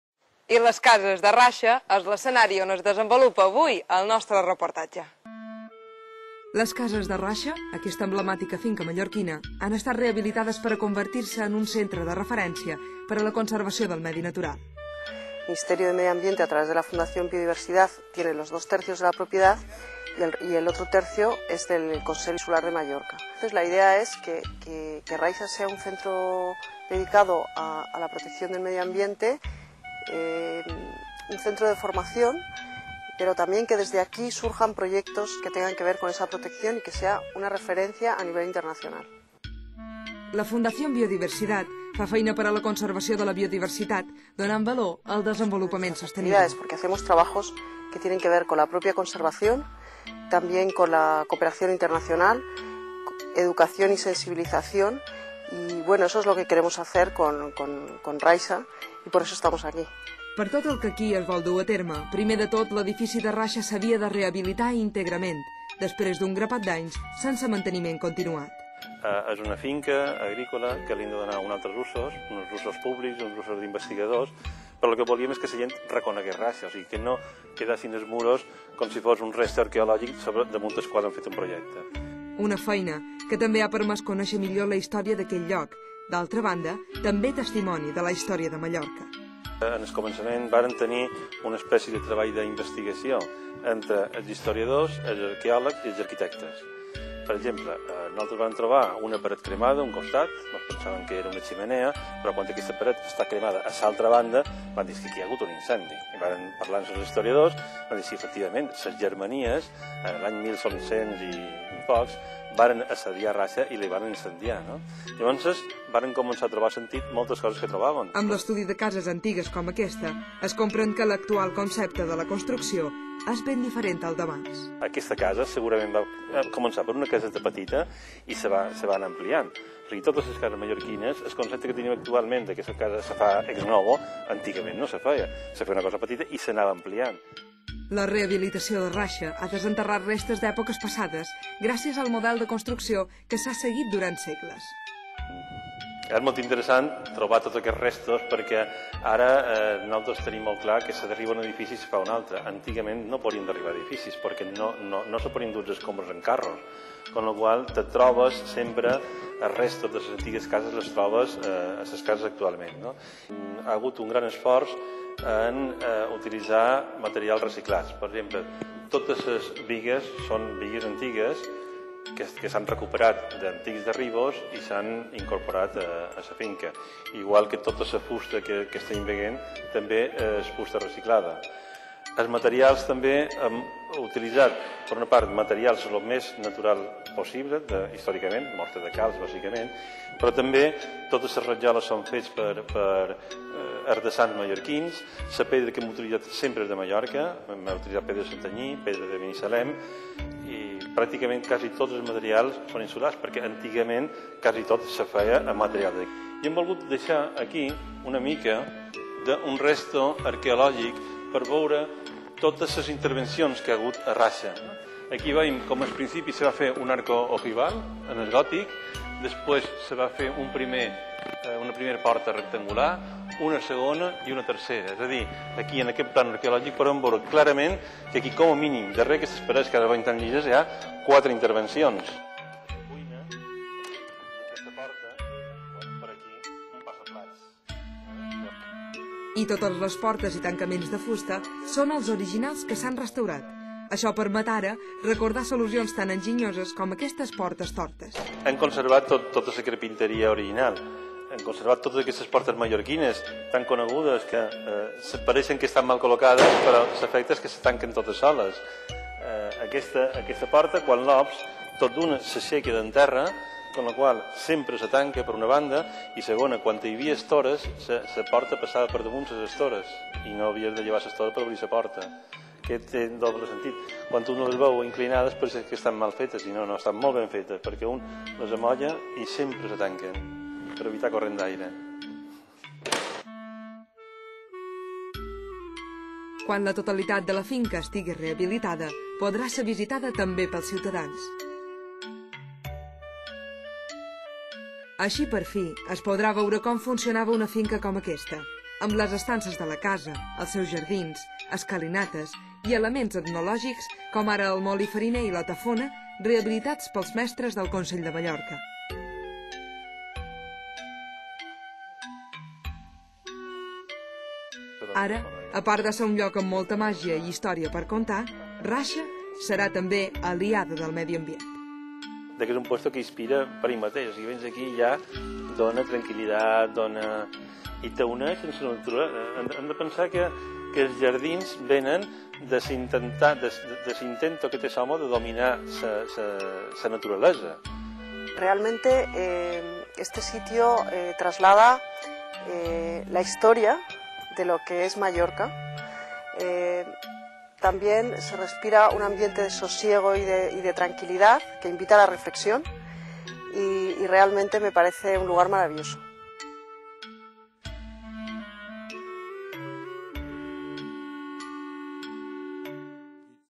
Cristina Narbona presenta la finca Raixa (2)
La ministra de Medio Ambiente, Cristina Narbona, junto a la presidenta del Consell de Mallorca, Francina Armengol, presidió esta tarde la presentación de la Finca Raixa como centro medioambiental.